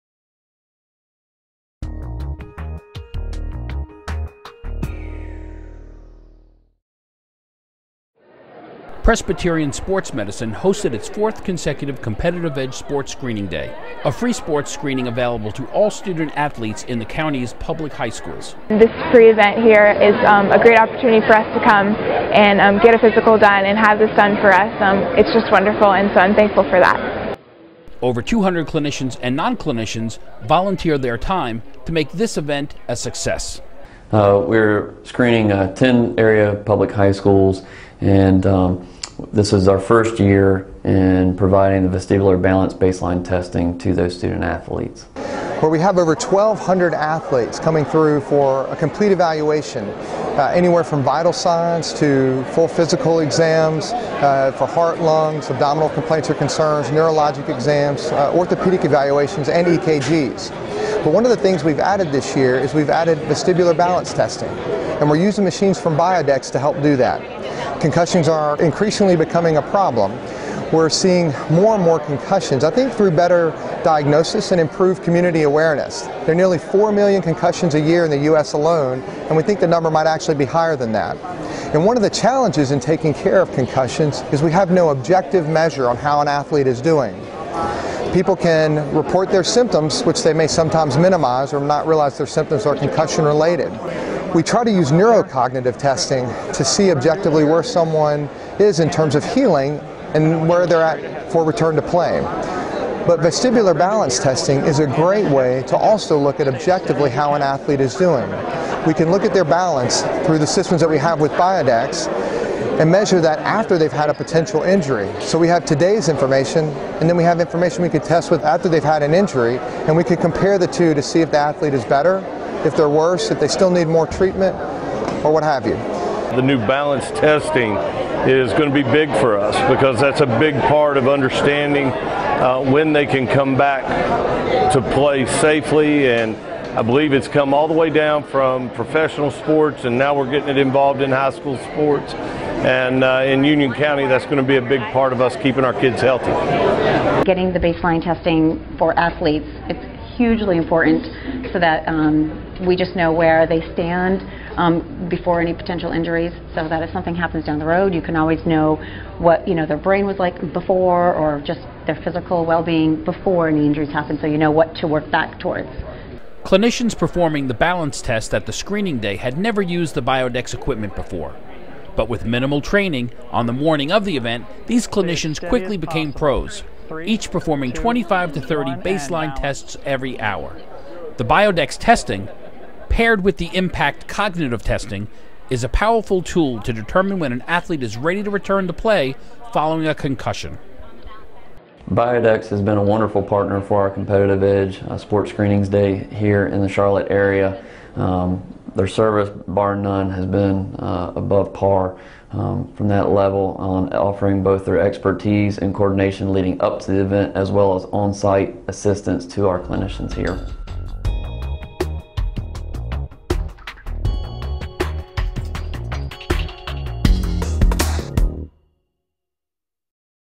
Biodex Balance Assessment Program for Concussion Management – Presbyterian Sports Medicine
Using the BioSway Presbyterian Sports Medicine introduced the Biodex Balance Assessment Program at their Fourth Annual Competitive Edge Sports Screening Day. Over 200 clinicians and non-clinician volunteer their time and offer a free sports screening for students in the county's High Schools.